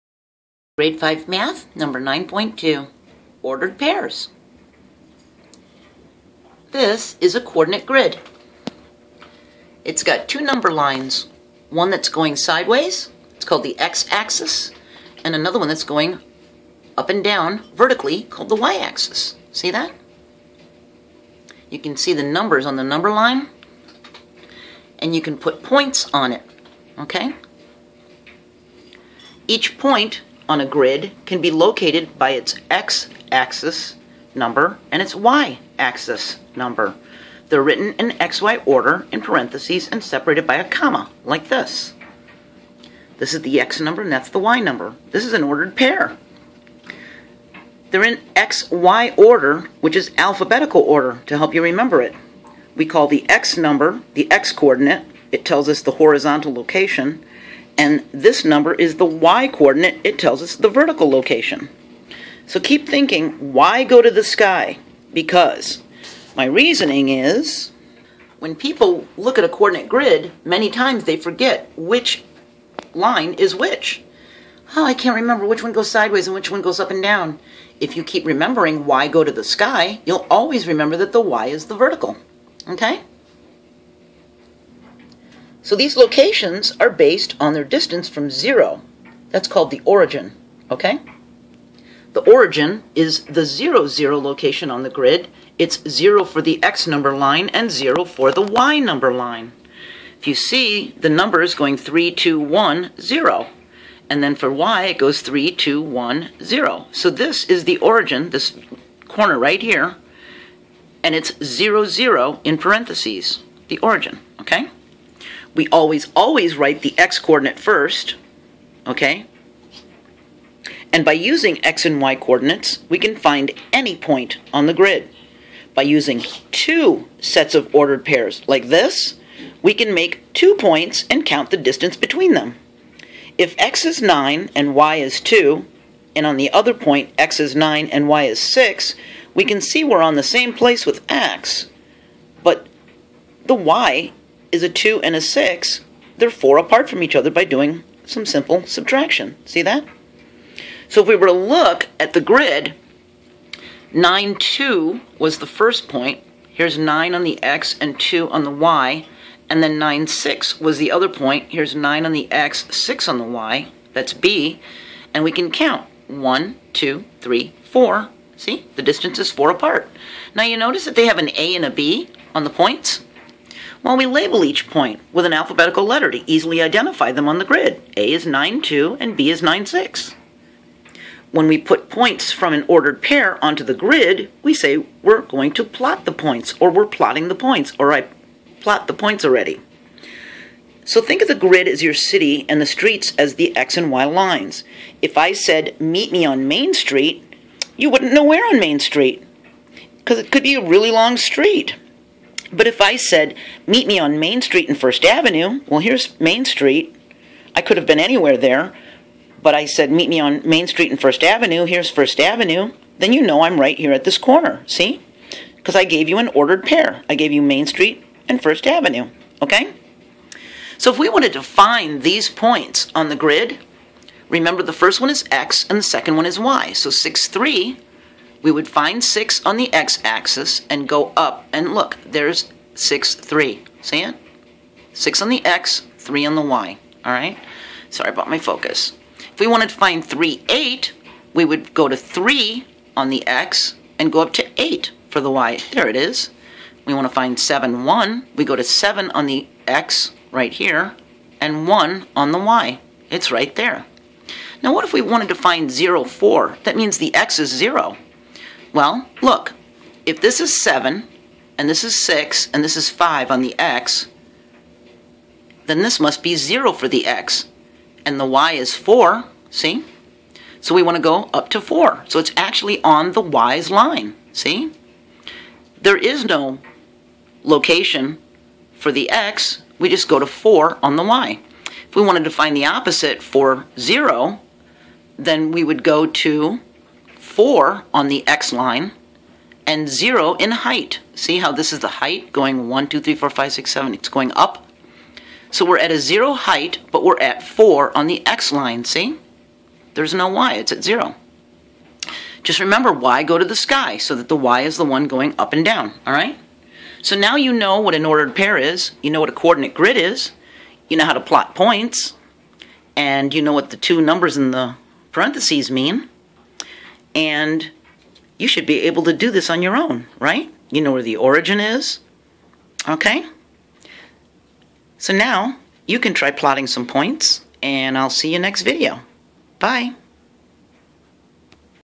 Grade 5 Math 9.2, Ordered Pairs, Coordinate Grid, X and Y Axis
Learn about a coordinate grid, it's x and y axis and how to remember which is which, ordered pairs, how to read a grid and how to plot points. #9.2

If you like my work and my videos are helping you,
Every dollar helps! Thank You!